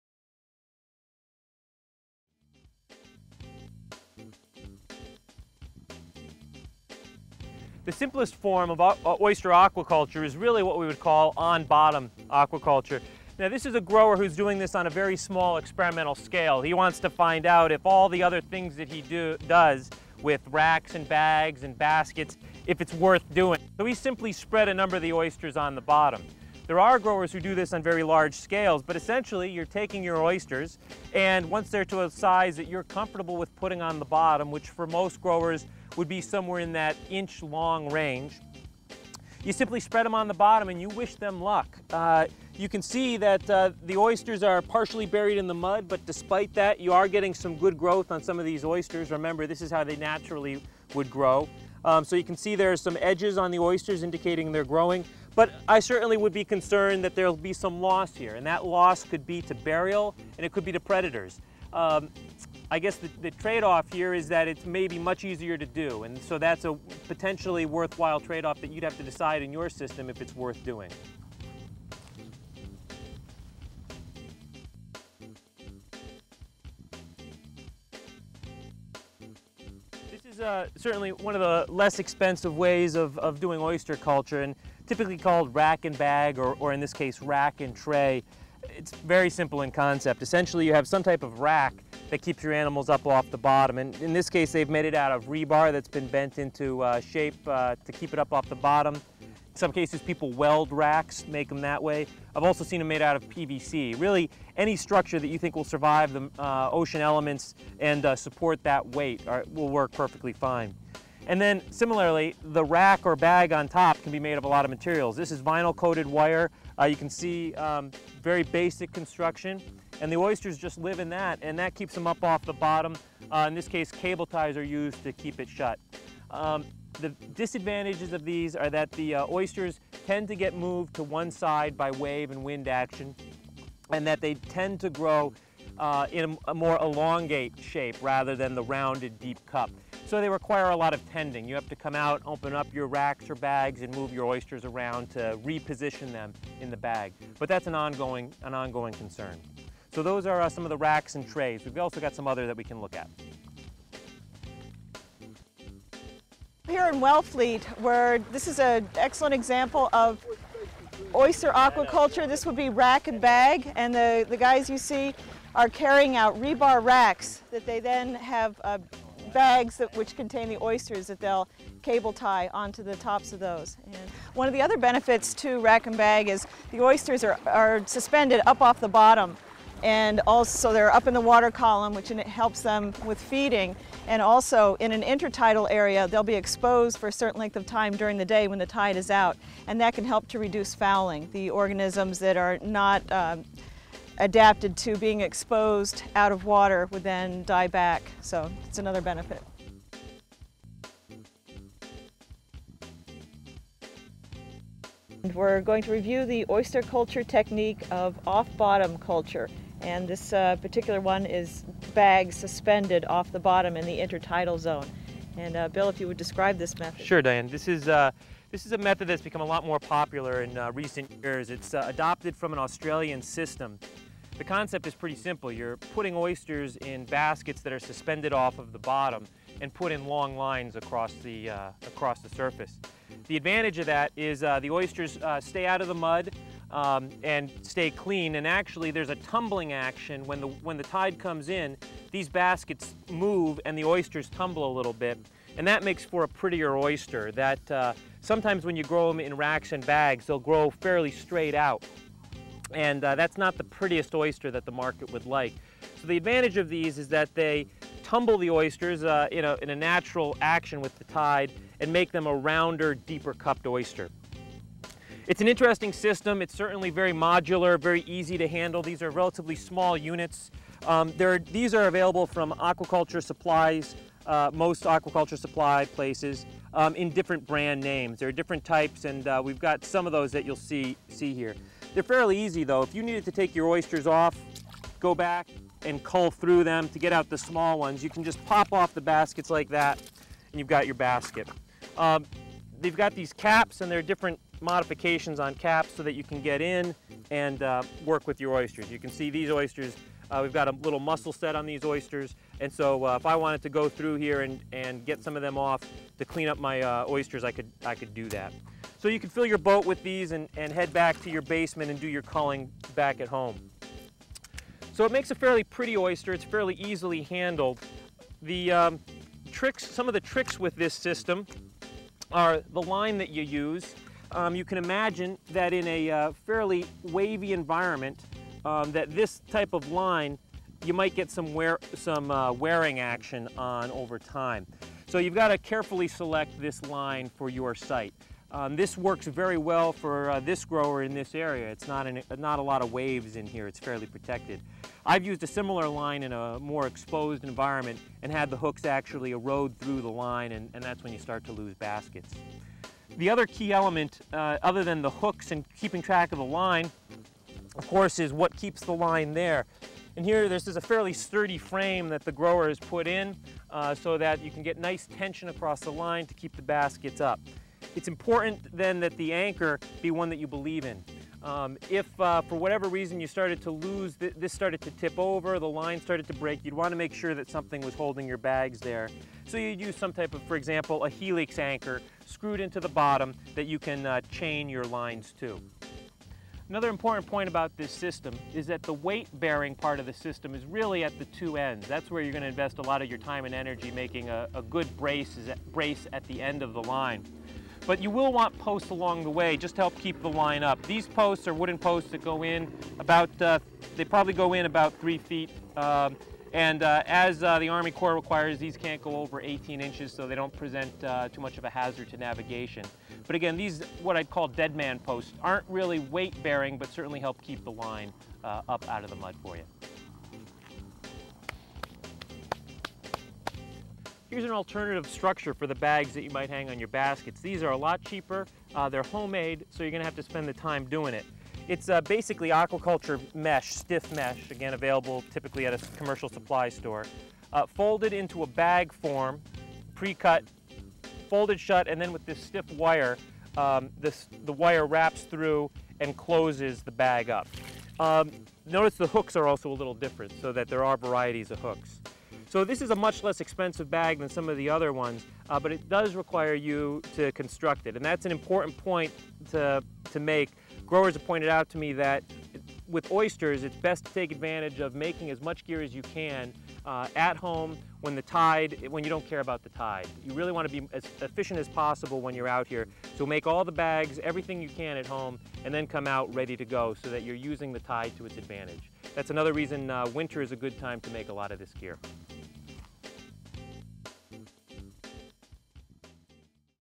Shellfish Aquaculture: Tools, Tips and Techniques
Just a taste of our video highlighting shellfish aquaculture tools, tips and techniques.